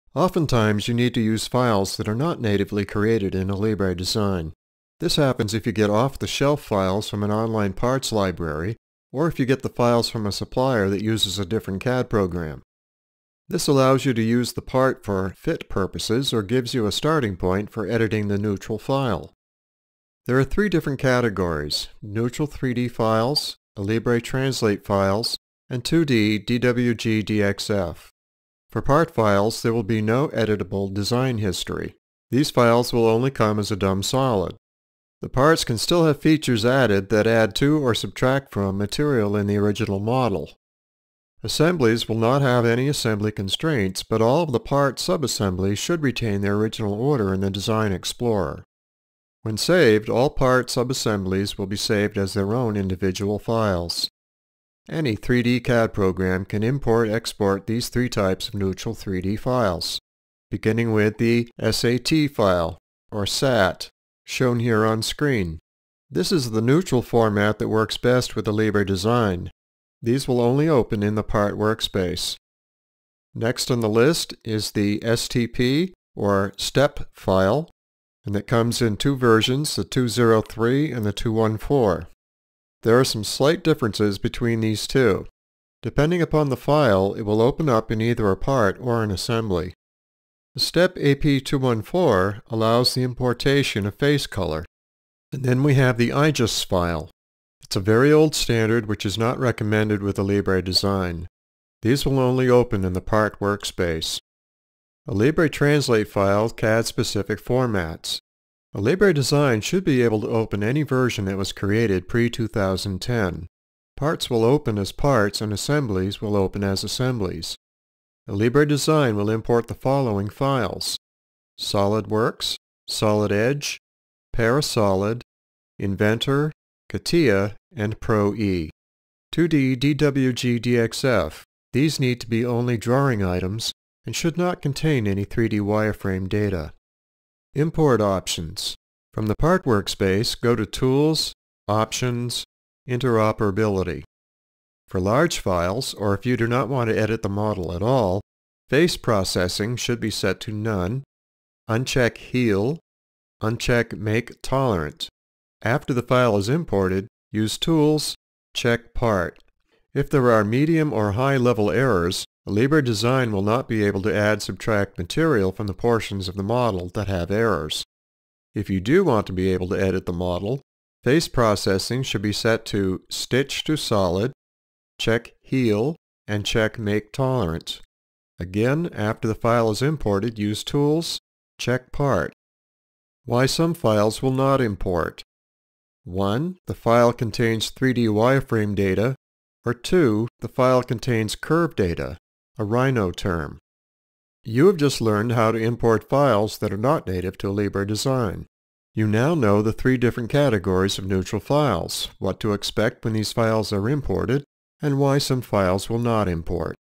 Importing Neutral Files in Alibre Design (Legacy Interface)
Although this video features an older version of Alibre Design from the mid-2000s, it offers valuable insights on importing CAD files in neutral formats. Learn handy tips and best practices for working with these universal file formats.